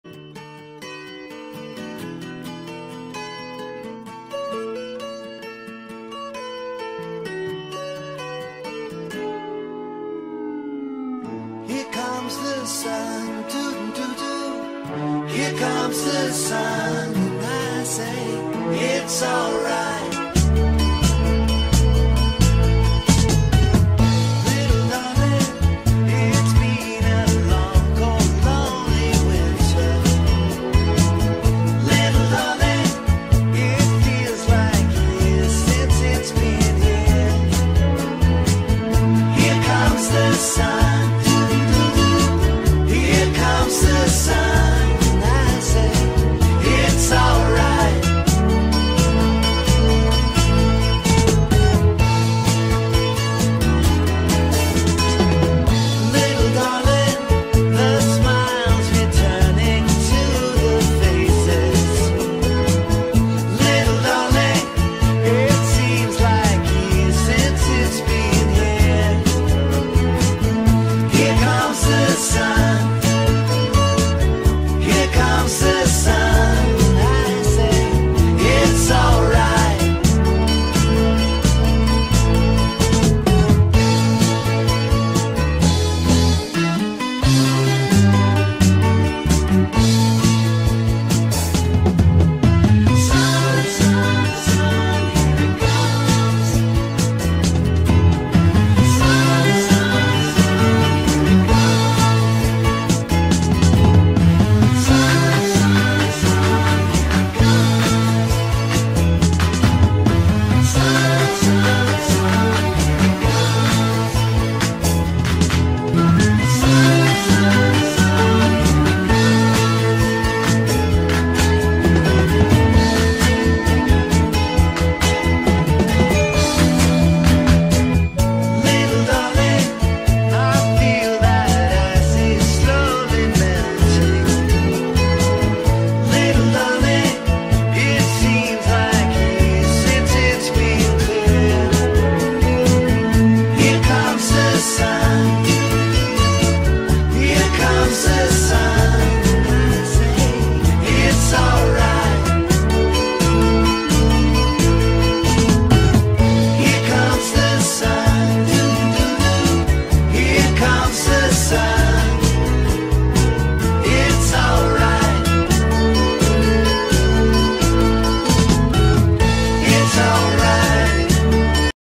A trip to the field! Happy new year! fieldtrip newyear #2022
Toby enjoyed his last day on the field in 2021! :)